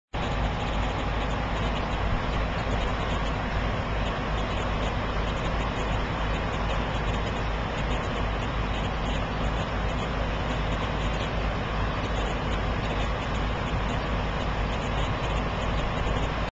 A little freightliner goes a long way.
1994 Freightliner FLD112. My little freightshaker at the Gebbers Farms dock. Brewster, WA. I've had this truck since November 1999.